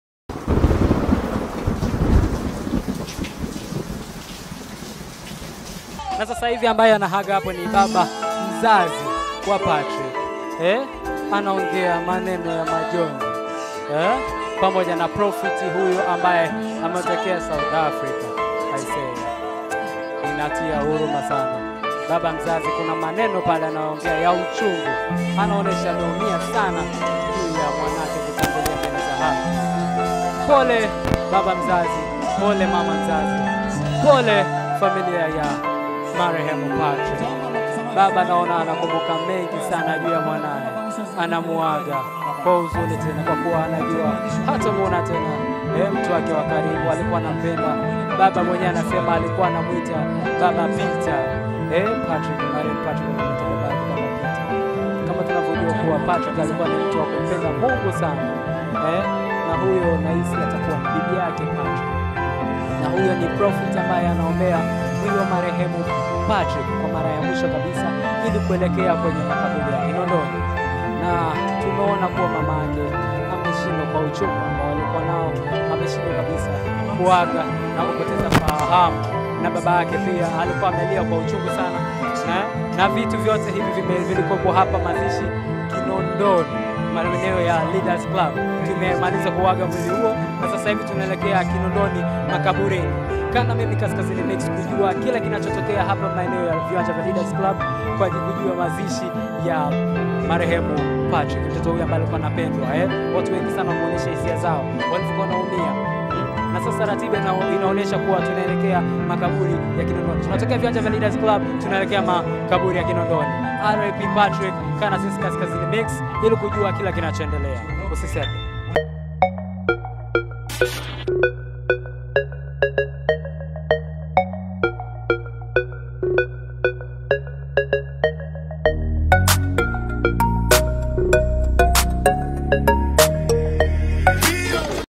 MAZISHI YA PATRICK | Baba Mzazi wa Patrick Peter akitoa heshima za Mwisho kwenye Jeneza la Mwanae
hatua za mwisho za kumuaga mtoto patrick wazazi wake washindwa kujizuia na kumwaga machozi mbele ya mwili wa mwanae